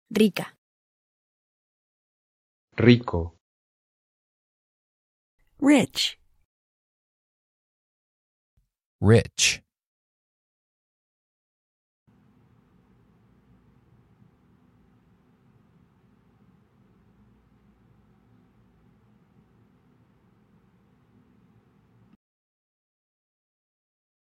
How to say "Rich" in Spanish
Male Voice:
Rico

Female Voice:
Rica

Male Voice Translation:
Rich

Female Voice Translation:
Rich